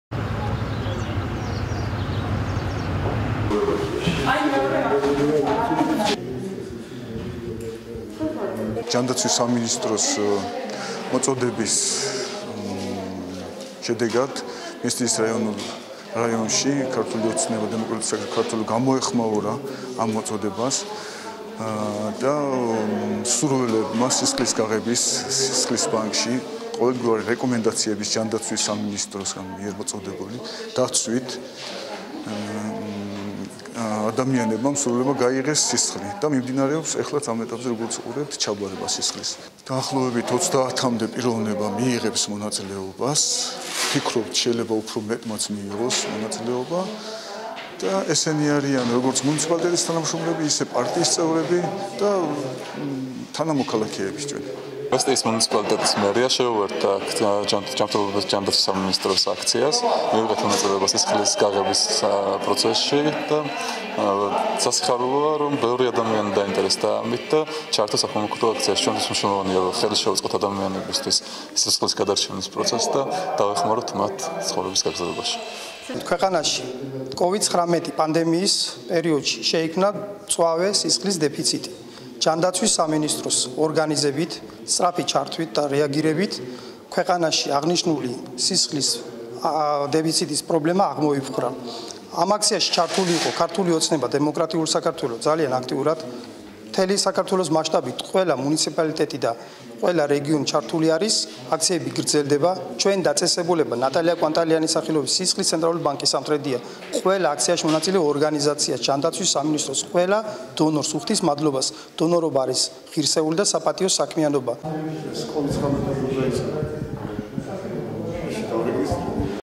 მესტიის მუნიციპალიტეტში სისხლის უანგარო დონაციის აქცია მიმდინარეობს
“გაიღე სისხლი – გადაარჩინე სიცოცხლე“ – მესტიის მუნიციპალიტეტში სისხლის უანგარო დონაციის აქცია მიმდინარეობს